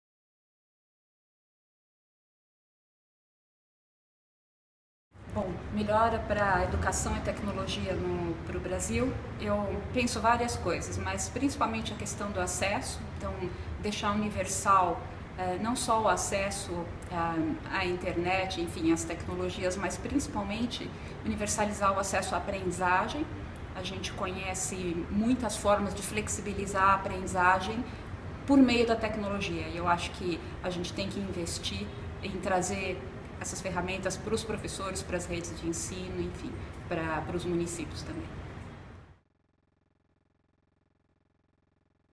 Monica Andrade Weinstein e as possibilidades da tecnologia no avanço da Educação
Monica Andrade Weinstein, diretora-técnica da Coruja Educação, falou sobre o uso da tecnologia como possibilidade para o ensino e aprendizagem no 6º Happy Hour da Educação.